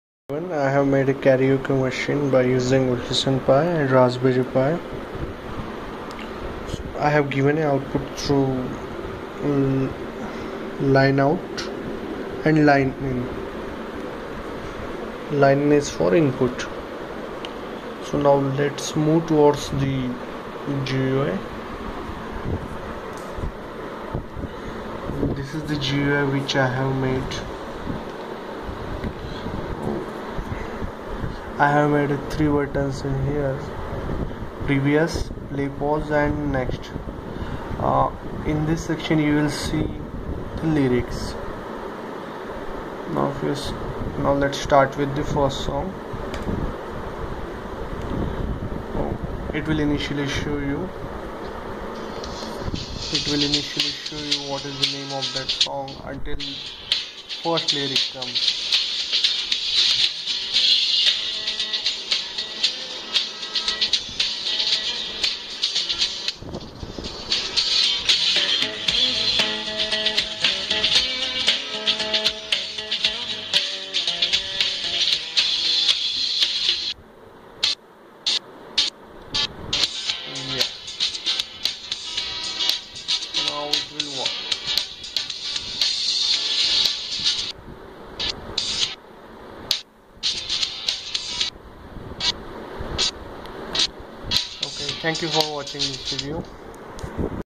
Karaoke Machine With Raspberry Pi and Wolfson PI.
I have tried to make a Karaoke Machine with Raspberry and Wolfson Pi.

I have also posted blog a detailed procedure and source code of project at my blog at Element14.